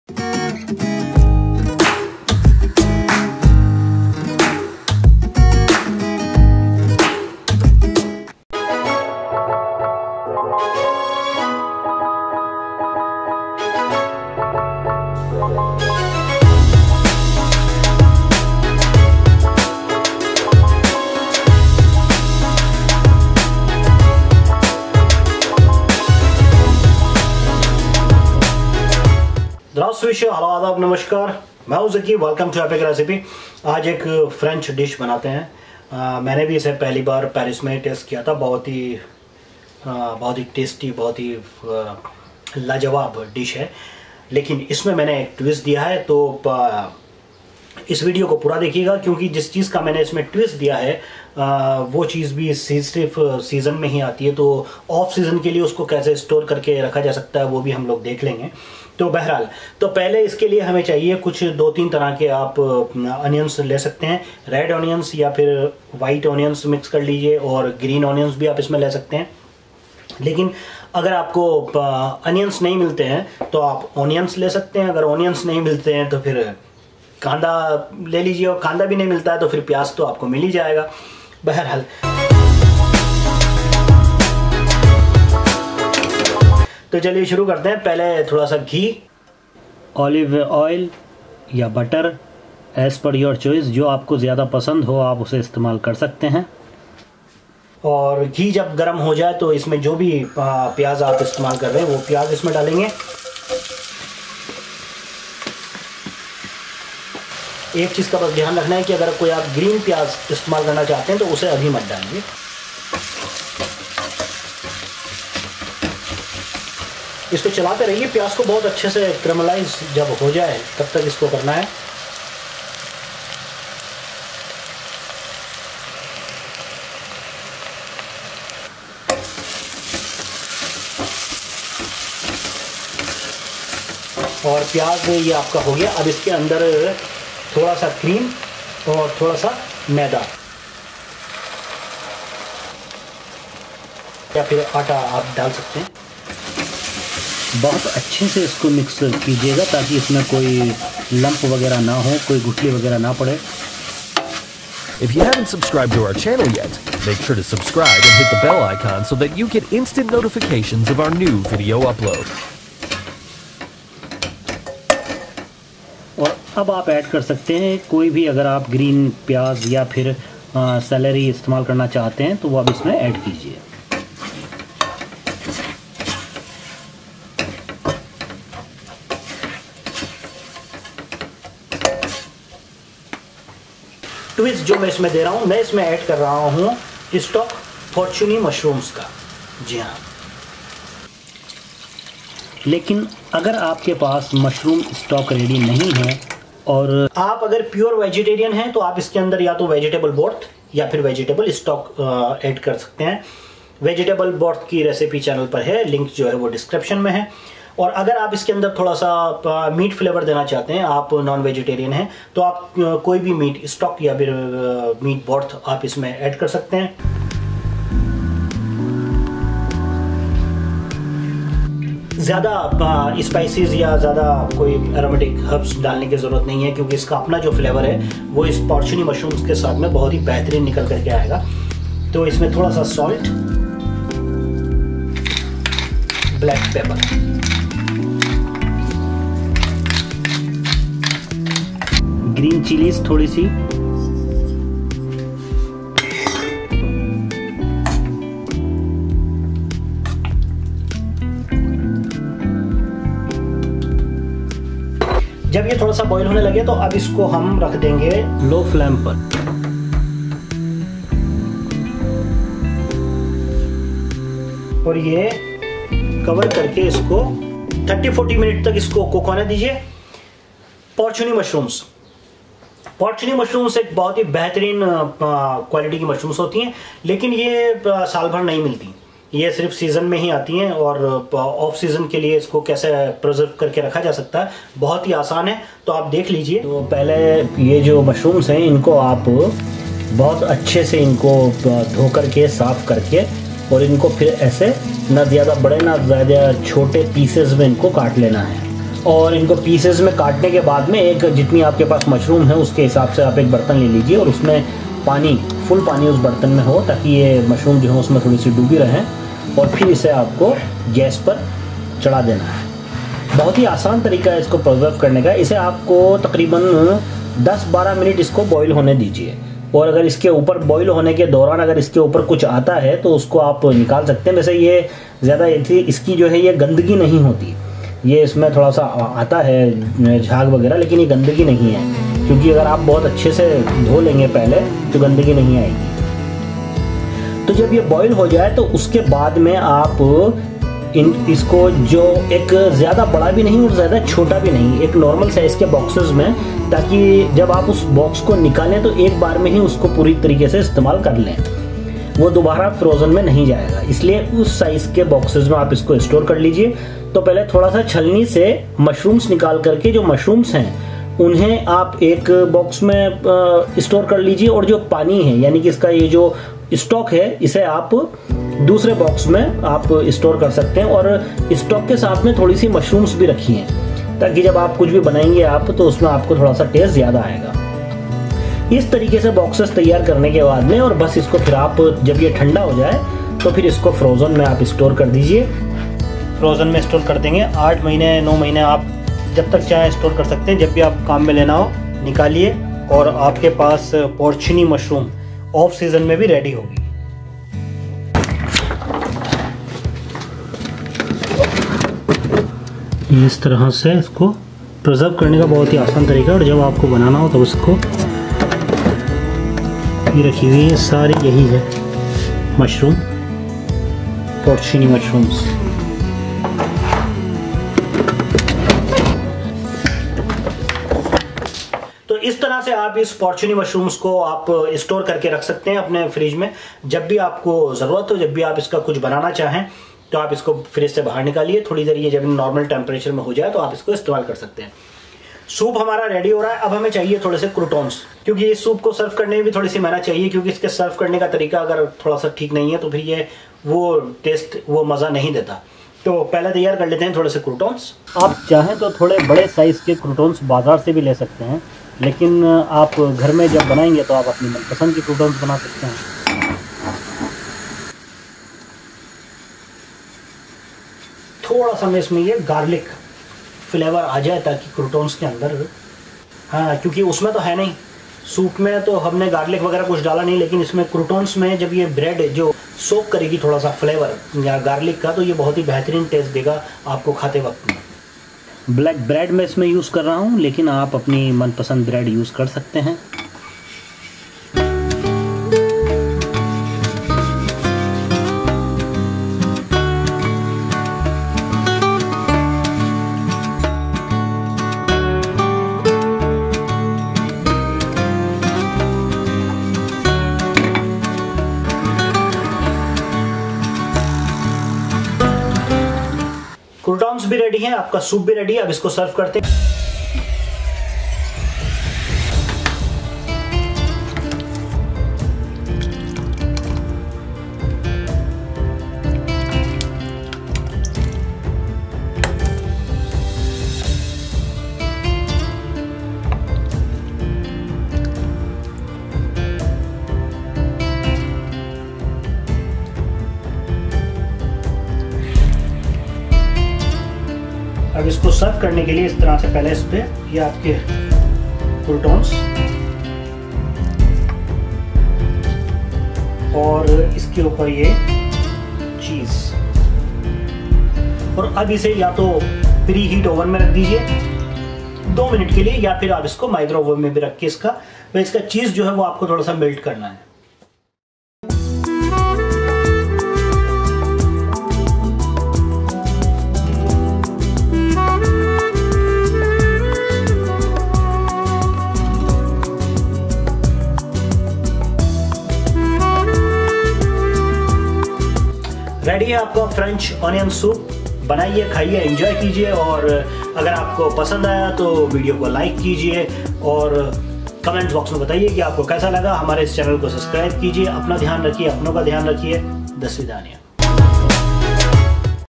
Authentic French Onion Soup Recipe with Porcini Mushroom Broth | Easy & Delicious Epic Recipe
Learn how to make the perfect French Onion Soup with a delicious twist—Porcini Mushroom Broth! In this easy-to-follow recipe, we’ll guide you through creating a rich, savory French Onion Soup, elevated by the earthy depth of porcini mushrooms. This comforting soup is the ultimate meal for any occasion, perfect for cozy evenings and family gatherings.

🍲 What You'll Learn in This Video:

How to make traditional French Onion Soup from scratch.
The secret to making a rich Porcini Mushroom Broth for extra flavor.
Tips for caramelizing onions to perfection.
Step-by-step instructions for an unforgettable homemade soup.
Ingredients You'll Need for French Onion Soup with Porcini Mushroom Broth:

Fresh onions
Porcini mushrooms
Vegetable or beef stock
Cheese for topping
And more!
This recipe is easy, comforting, and full of flavor—whether you’re cooking for a special occasion or simply enjoying a hearty meal at home.